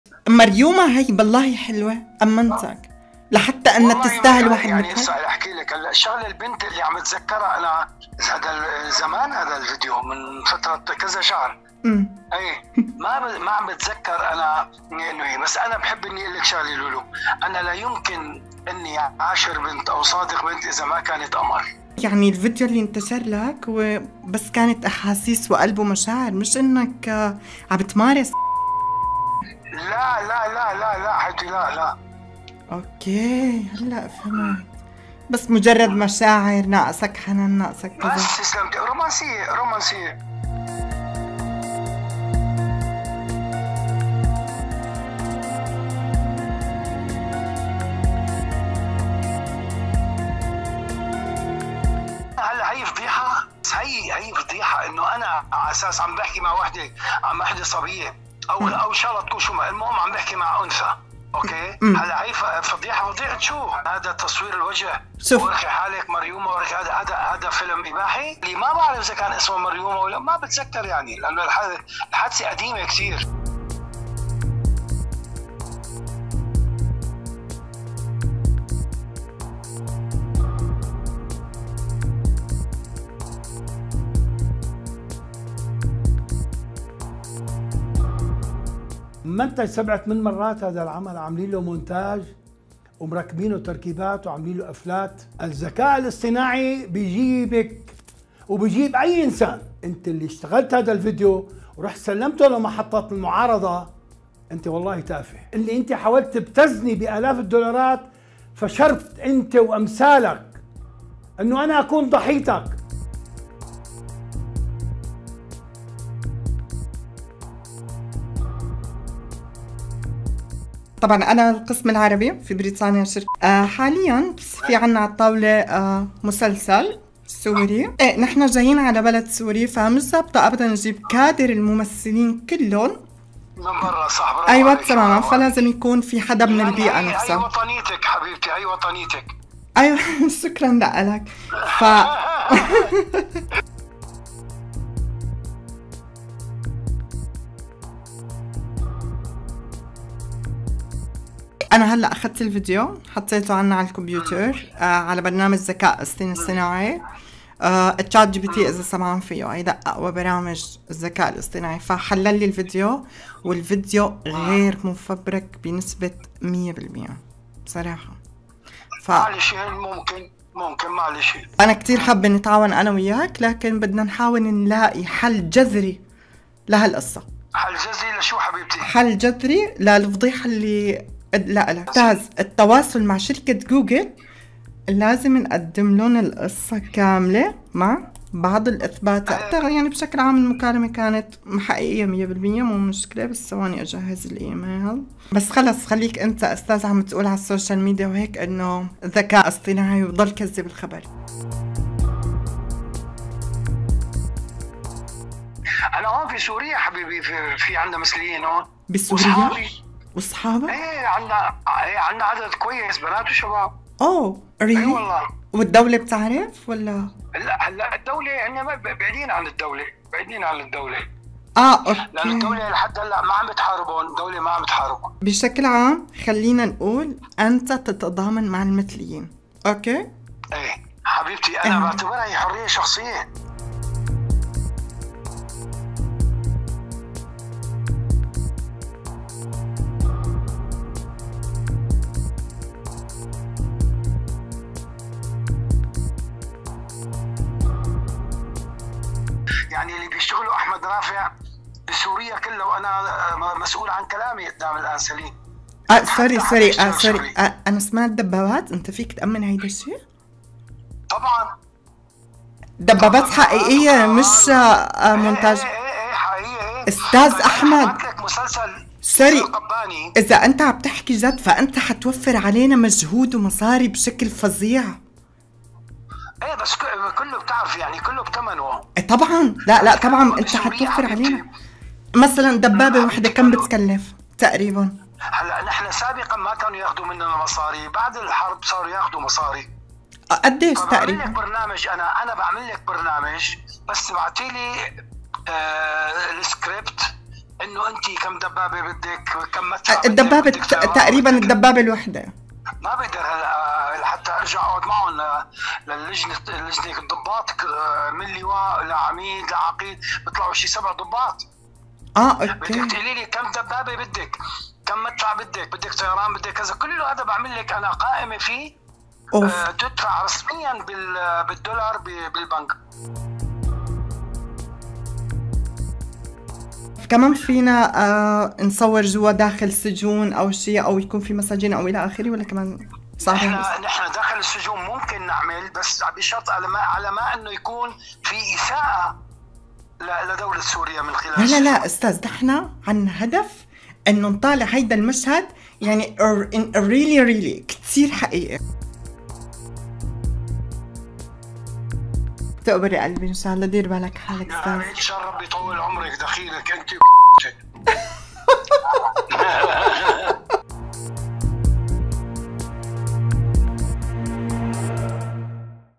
الممثل أحمد رافع يقع في فخ يوتيوبر سوري
كشف تفاصيل وحقائق عن الفيديو الجنسي مع "مريومة"
ووضع ضباط بقوات الأسد في "ورطة"